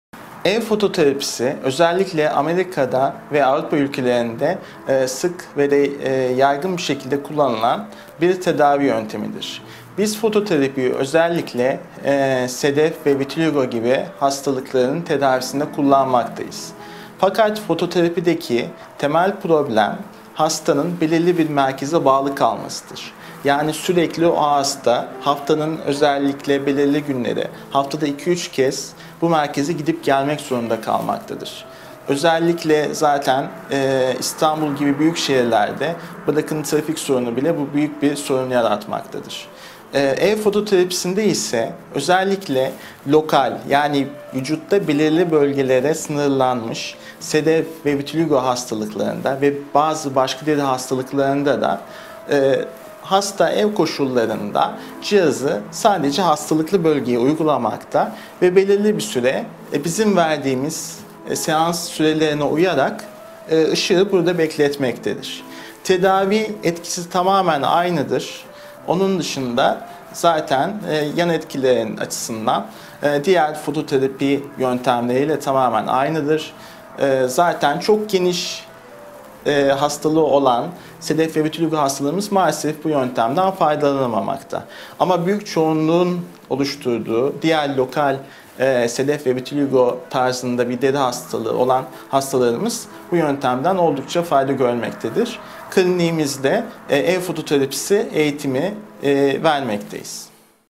Fototerapi nedir, ne zaman uygulanır?
Fototerapi nedir, ne zaman uygulanır? başlıklı video, Deri ve Zührevi Hastalıklar uzmanı Uzm. Dr. Sertaç SEVER tarafından cevaplanıyor.